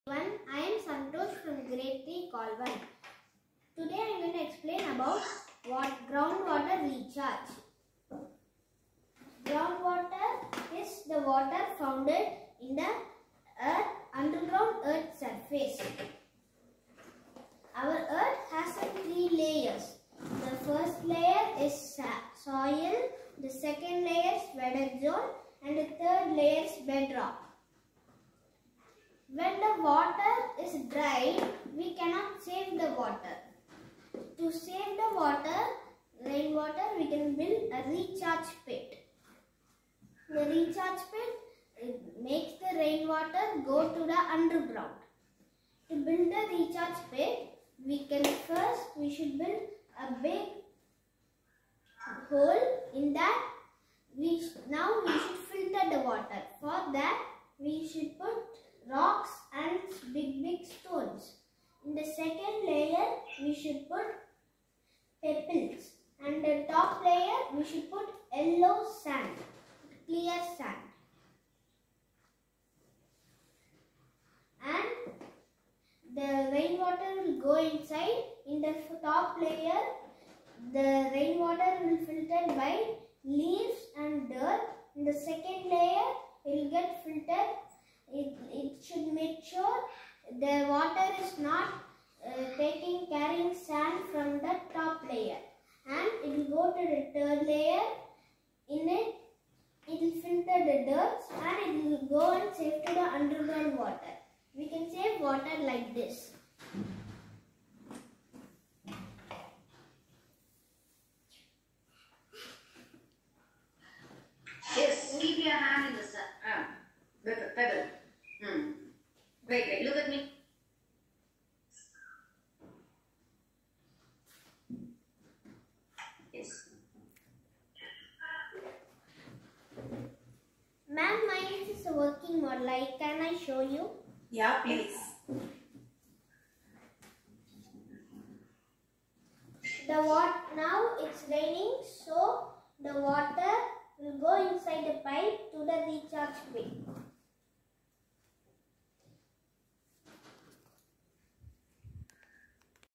Kids Project Idea - Groundwater Recharge - Working Model
Kids school project idea on ground water recharge with working model.
By Santhosh (My Nephew)